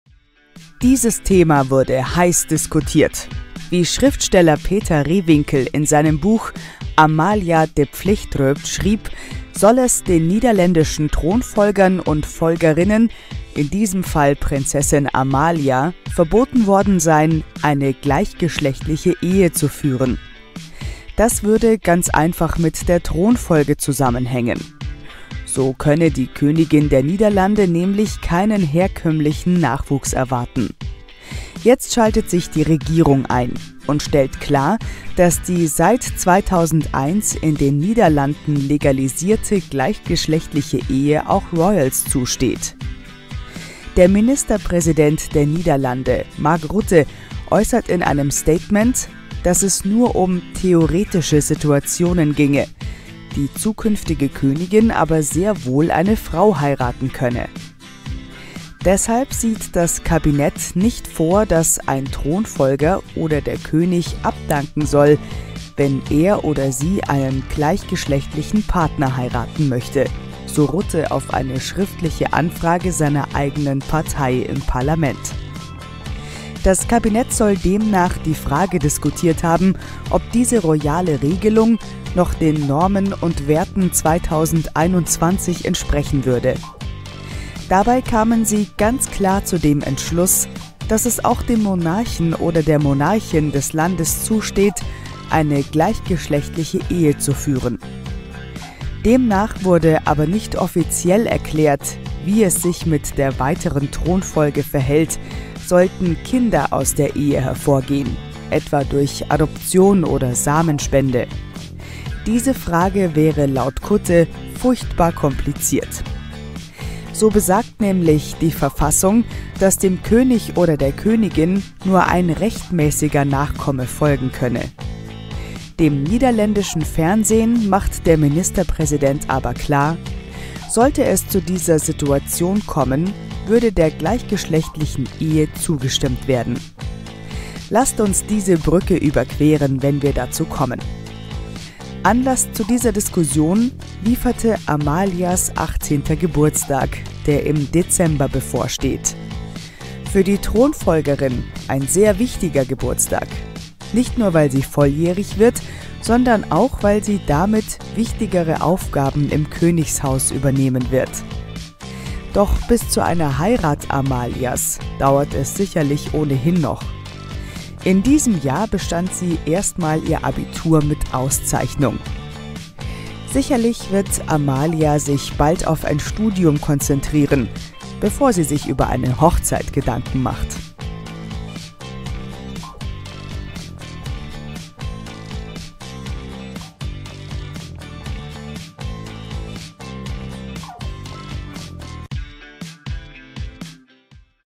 Streit mit der Regierung! Darf Prinzessin Amalia eine Frau heiraten? • PROMIPOOL
Darf Prinzessin Amalia eine Frau heiraten oder nicht? Der niederländische Royal ist Thronfolgerin, was eine gleichgeschlechtliche Ehe ausschließen würde. Oder etwa doch nicht?